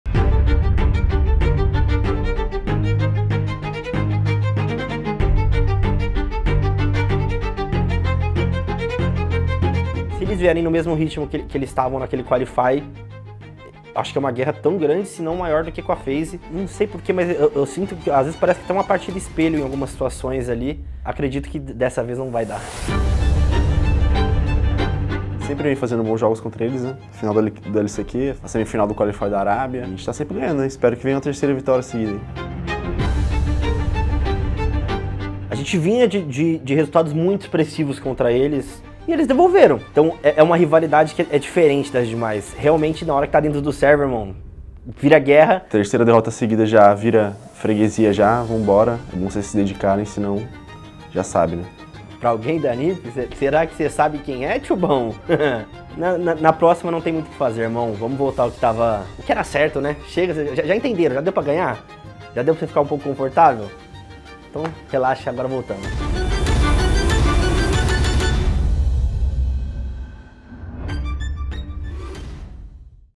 PRÉ-JOGO: LOS X NINJAS IN PYJAMAS I BLASTR6: LIGA BRASILEIRA - STAGE 2 I Rainbow Six Siege
Confronto do Playday #2, confira as expectativas das equipes para LOS x Ninjas in Pyjamas na BlastR6: Liga Brasileira.